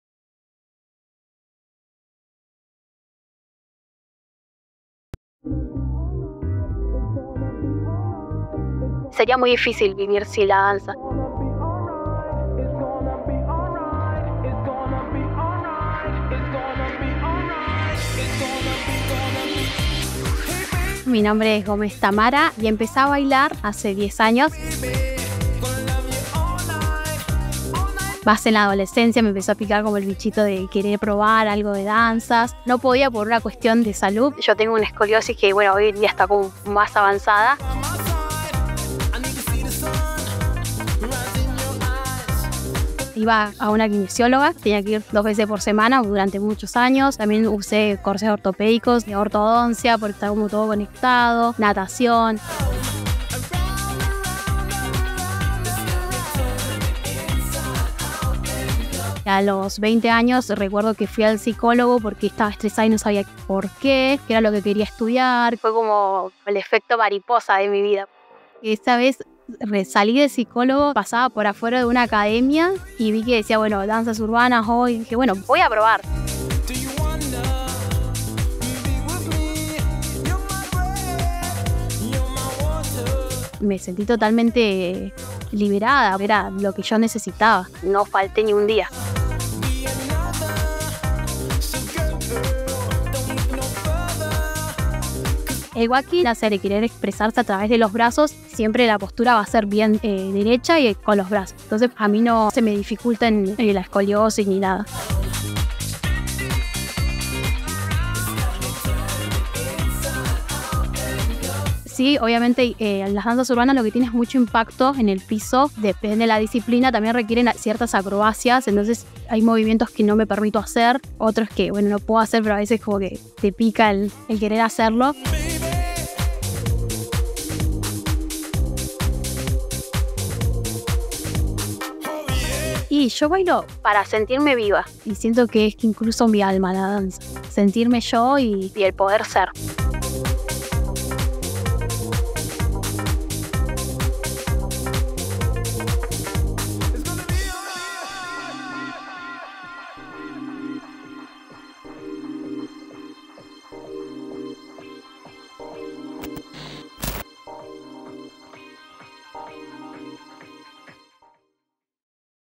MOTUS 7: Tami Gómez
Conocemos a una bailarina de "wwking" en el 7° episodio de Motus.